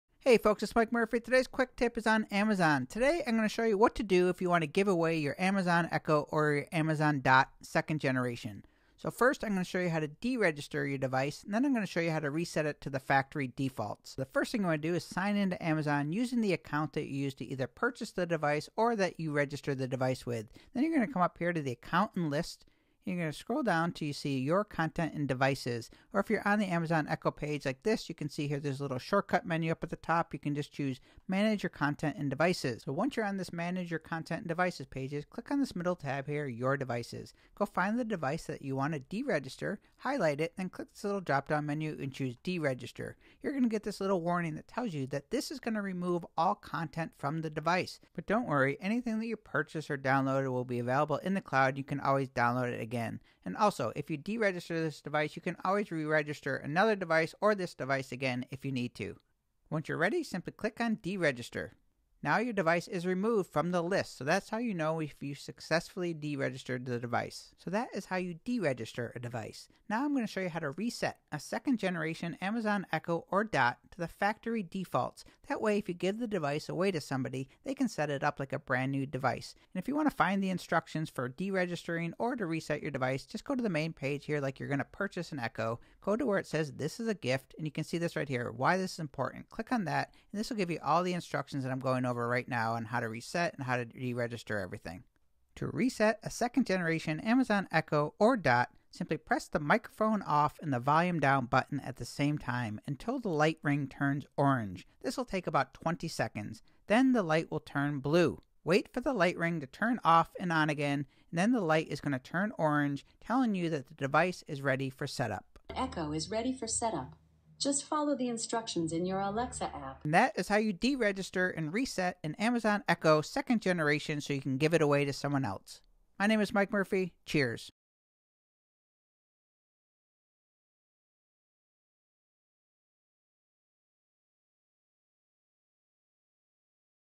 How to Reset Amazon Echo or Dot (2nd Gen) to Factory Defaults
Step 1: Deregister
Log in to to Amazon
Click on your Name...Accounts & Lists (where you manage things)
Scroll down and click 'Your Content & Devices'
Click on the Middle Tab, 'Your Devices'
Click on the Device
Click the Dropdown menu and select 'Deregister'

Step 2: Reset Amazon Echo or Dot (2nd Gen):
With device plugged in

Press and hold the Microphone off and Volume down buttons at the same time until the light ring turns orange (about 20 seconds).

The light ring turns blue.

Wait for the light ring to turn off and on again. The light ring then turns orange again, and your device enters setup mode.

The device is ready for setup with the Amazon Alexa App